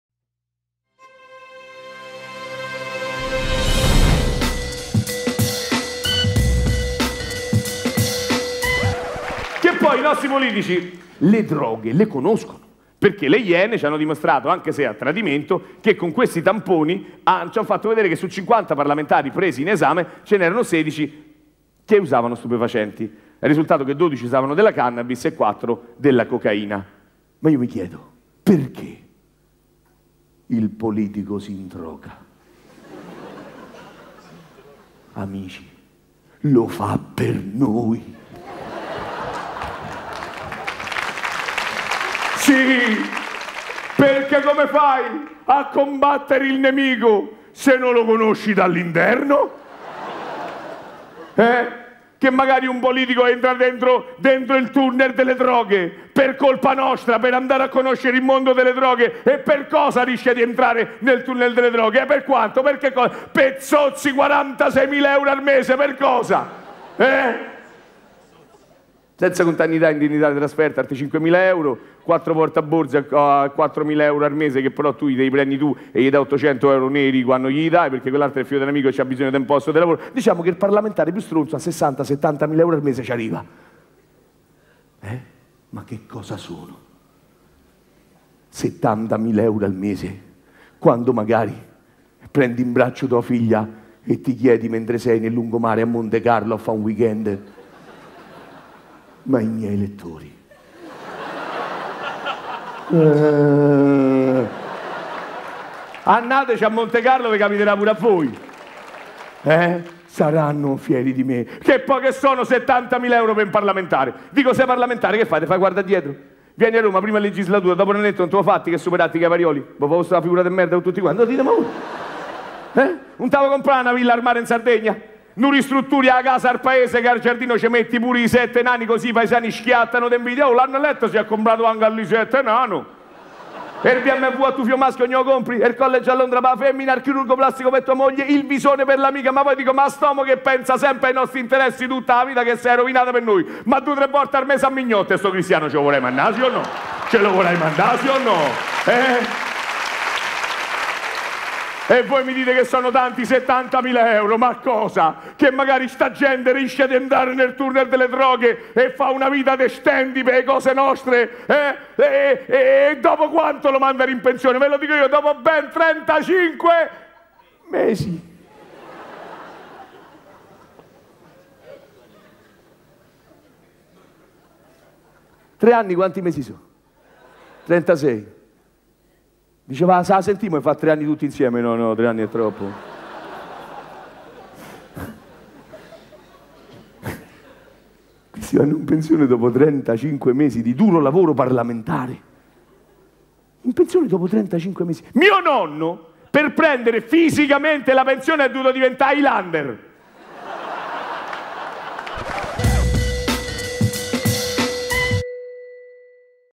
Max Giusti: Perchè un politico si droga?
Max Giusti mostra il suo volto oltre le parodie: quello di un uomo di 38 anni, un artista, ma soprattutto una persona semplice che si gioca la faccia per raccontarsi nell’amore, nella politica, nei rapporti con gli altri.

Qualcosa in più sullo spettacolo: Max Giusti propone alcuni tra i suoi più deliziosi personaggi tra cui il flemmatico presidente laziale Claudio Lotito che Max da sfegatato romanista imita con sottile ribalderia. Aldo Biscardi, Claudio Lotito, Onorevole Mastella, Cofferati, Ricucci e tanti altri ospiti prendono parte allo spettacolo, per rispondere su temi di stretta attualità. Max racconta, nel suo modo scanzonato e tipicamente romano, anche lo smarrimento delle persone qualunque, di noi tutti, davanti alle vessazioni della burocrazia, alla cialtroneria della bassa politica, alla fatica del vivere quotidiano fatto di traffico, multe, tasse, parcheggi, insieme all’arroganza dei nuovi ricchi. E questa volta lo fa senza imitare alcun personaggio, ma come annuncia il titolo, mettendoci la faccia.
"Tutti siamo costretti a mettere la nostra faccia su ogni piccola decisione o evento della nostra vita".

Sacro, profano, politica e società, indecisioni piccole e grandi che caratterizzano le vite di ognuno di noi. L'importante è avere il coraggio di metterci sempre la propria faccia... o quella degli altri se hai la fortuna di essere Max Giusti.

Una produzione AB Management.
Di Stefano Fabrizi, Massimiliano Giusti, Paolo Mariconda
con MAX GIUSTI

SEGUITE MAX GIUSTI sui SOCIAL!